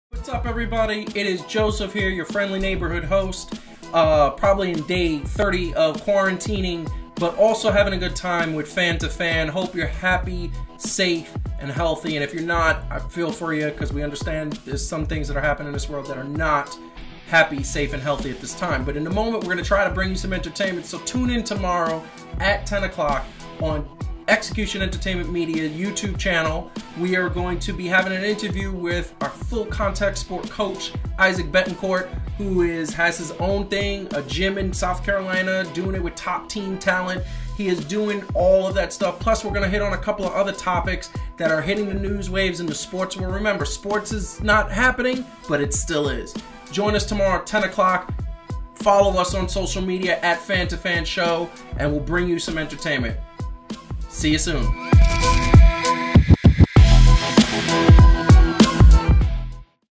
Fan2fan EP 12 Preview
Tomorrow Wednesday, April 15th at 10 pm we go live with a interview and topics such as XFL closing, WWE essential in Florida?, Dak throwing parties and so much more! Tune in and join the chat.

Also follow us @fan2fanshow on Twitter, Facebook and the Gram.